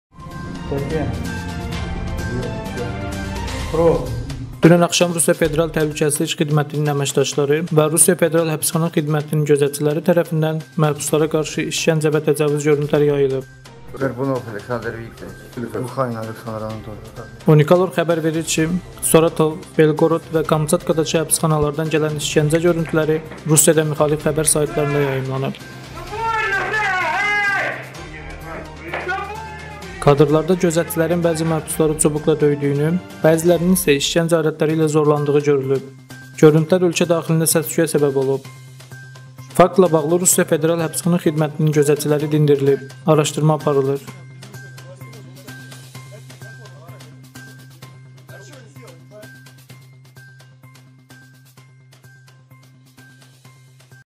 Rusiyada Dəhşətli Hadisə
Dünən axşam Rusiya Federal Təhlükəsizlik Xidmətinin (FSB) əməkdaşları və Rusiya Federal Həbsxana Xidmətinin (FSIN) gözətçiləri tərəfindən məhbuslara qarşı işgəncə və təcavüz görüntüləri yayılıb.

Unikal.org xəbər verir ki, Soratav, Belqorod və Kamçatkadakı həbsxanalardan gələn işgəncə görüntüləri Rusiyada müxalif xəbər saytlarında yayımlanıb.

Kadrlarda gözətçilərin bəzi məhbusları çubuqla döydüyünü, bəzilərinin isə işgəncə alətləri ilə zorlandığı görülüb. Görüntülər ölkə daxilində səs-küyə səbəb olub.

Faktla bağlı Rusiya Federal Həbsxana Xidmətinin gözətçiləri dindirilib. Araşdırma aparılır.